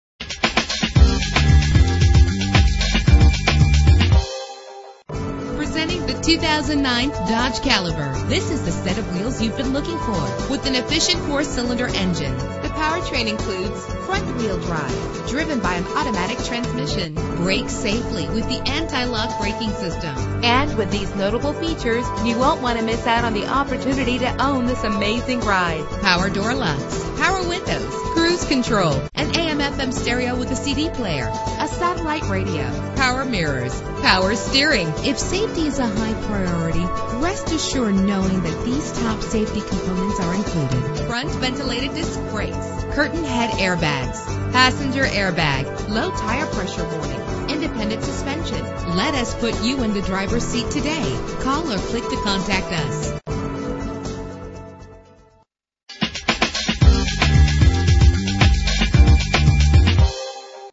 2009 Dodge Caliber - Stafford TX
Year: 2009
Make: Dodge
Model: Caliber
Trim: SXT
Engine: 2.0 liter inline 4 cylinder 16 valve
Transmission: Automatic
Color: Bright Silver Clearcoat Metallic
Mileage: 65377
Address:
12053 Southwest Freeway